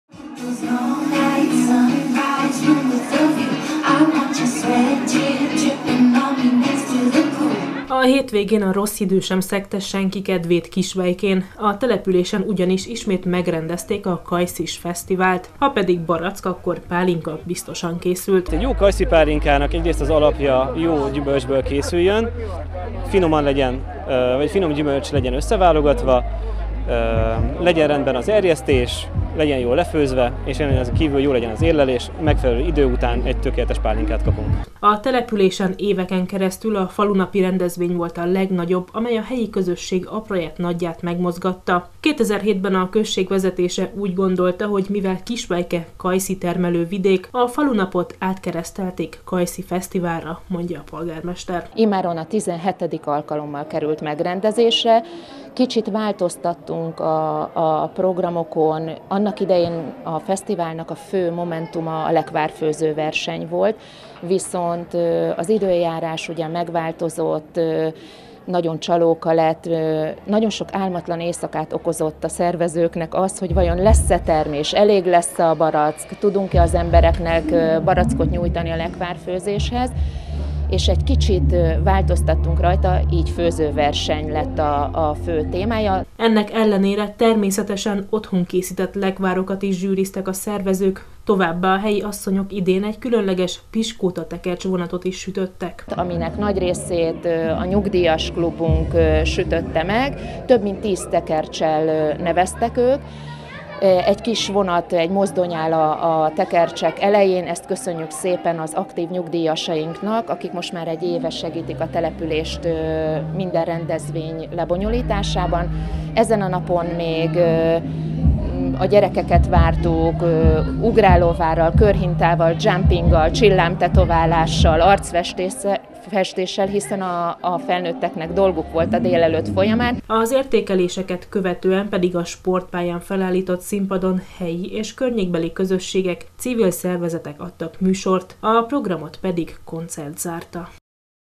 Kajszis Fesztivál Kisvejkén
Idén is megtartották Kisvejkén a sokak által jól ismert, már hagyománynak számító Kajszis Fesztivált. A délelőtt során a látogatók lekvár-, pálinka- és bográcsfőző versenyre is benevezhettek, emellett különleges piskótatekercs vonat is készült. Délután pedig kulturális programok és koncert szórakoztatta a résztvevőket.